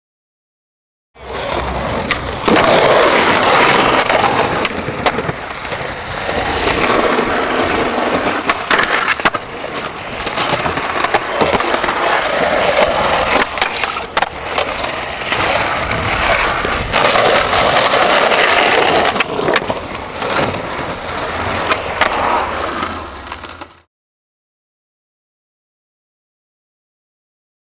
sausage skateboards in Bellingham
the sausage skate crew came up to Bellingham and ripped. Heres a tease of a edit coming soon.